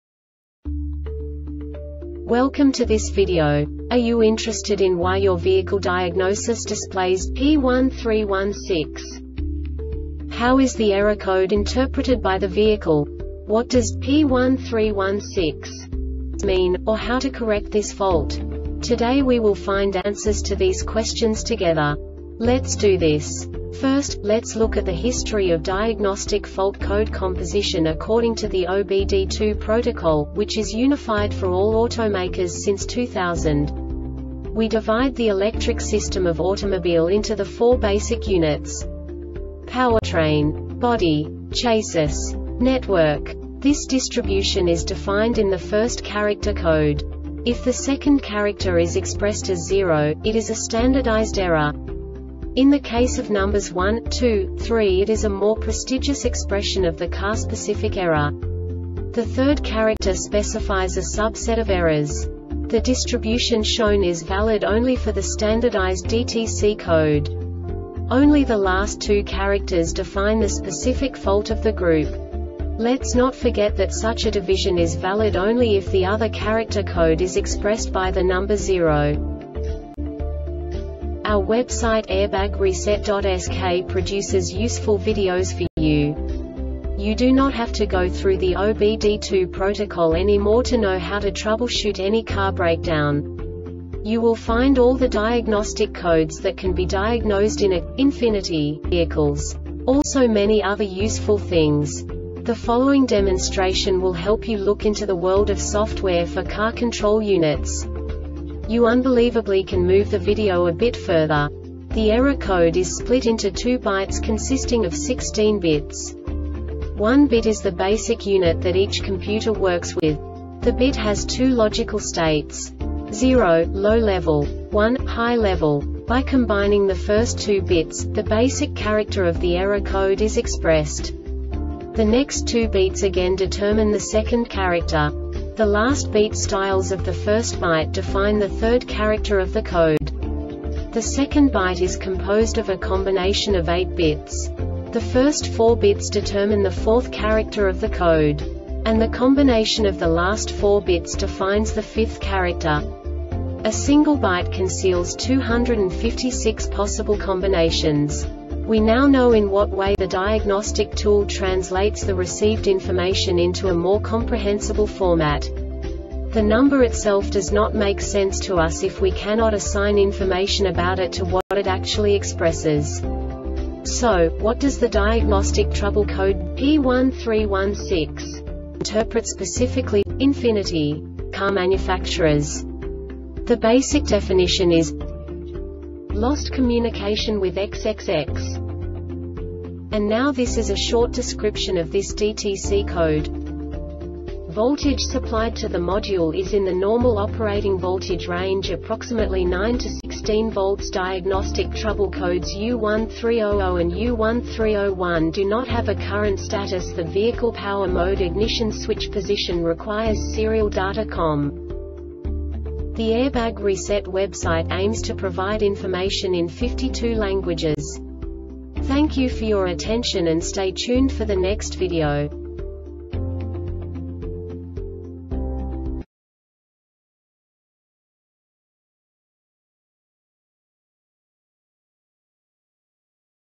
DTC Ford P1316 Short Explanation
Contents:
0:21 Basic DTC analysis according to OBD2 protocol standard.
1:48 Insight into programming
2:58 A diagnostically understandable description of the Diagnostic Trouble Code
3:05 Basic error definition

Make:
Ford
Code:
P1316
Definition:
Lost Communication with XXX
Description:
Voltage supplied to the module is in the normal operating voltage range (approximately 9-16 volts).Diagnostic trouble codes U1300 and U1301 do not have a current status.The vehicle power mode (ignition switch position) requires serial data com